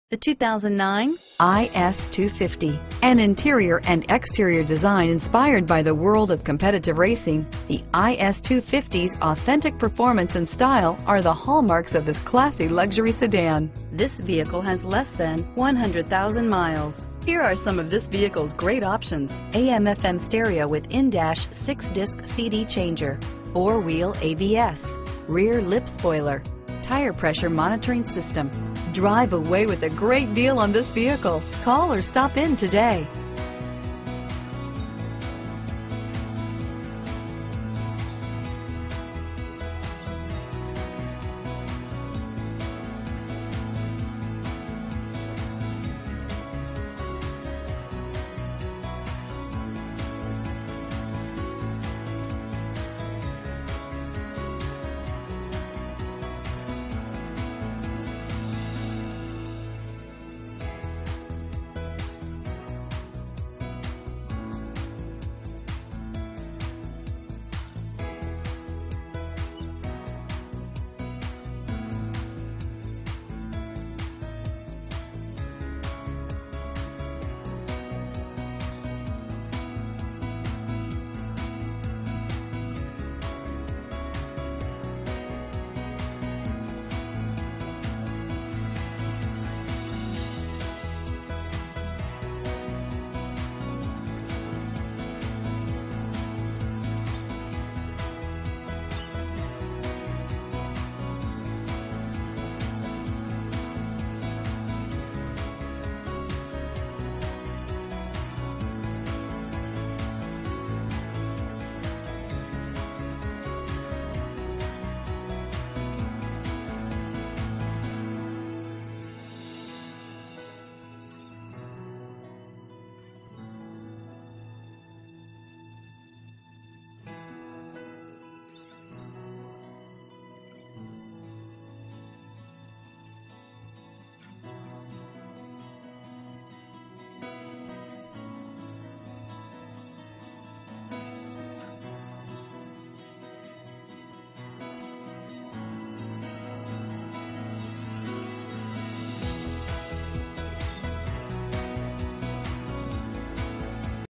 2009 Lexus IS 250 San Francisco, San Jose, Oakland, Marin, bay area, CA 35891
2009 Lexus IS 250

PREMIUM PKG -inc: perforated leather seat trim dark gray birds-eye maple or golden brown maple interior trim, REAR LIP SPOILER, STERLING PERFORATED LEATHER SEAT TRIM, Color-keyed heated pwr mirrors -inc: puddle lamps integrated turn signals|AM/FM stereo w/in-dash 6-disc CD changer -inc: MP3 194-watt amplifier (13) speakers auxiliary audio input automatic sound levelizer (ASL)|10-way pwr heated & ventilated front bucket seats w/pwr lumbar|Pwr front ventilated & rear solid disc brakes|Manual tilt/telescopic leather-wrapped steering wheel w/audio & cruise controls|4-wheel anti-lock brake system (ABS)|Tire pressure monitoring system